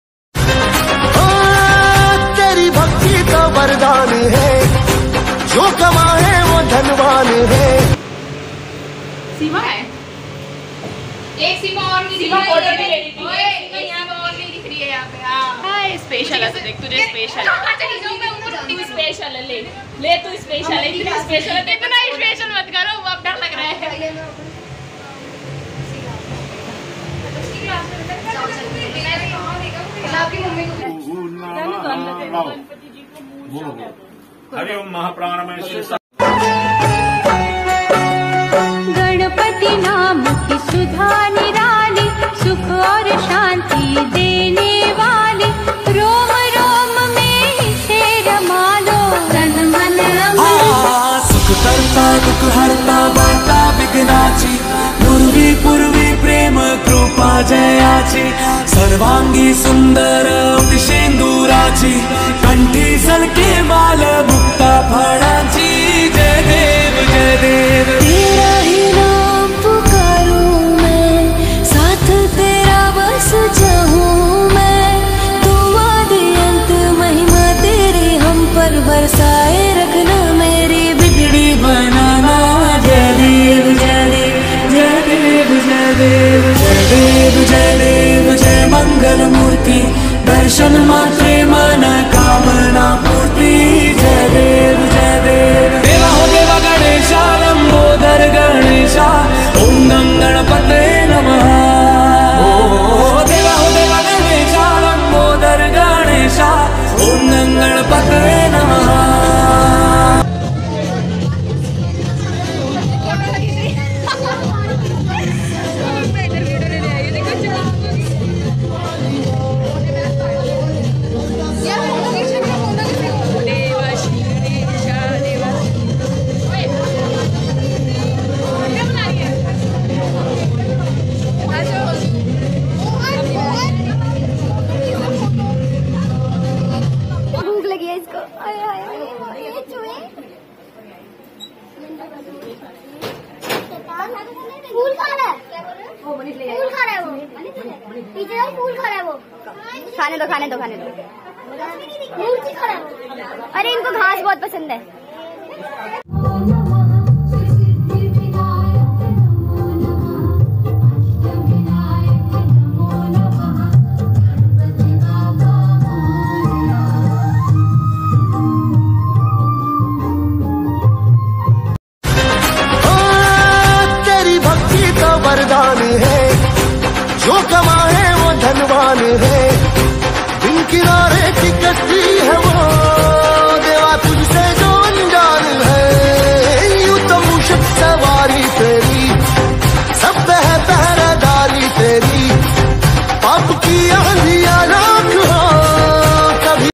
Part(3) Finally Face dekhne ko mila ganu ji ka..😍❤️Ganpati puja Special..
First Gnpati Puja/Special Moment in New house...
All about ganpati puja full vlog masti dhamal..bellow
life is like do all things with responsibility and fun...
Also on Mx Taka Tak
Rigels Together

Rigels Together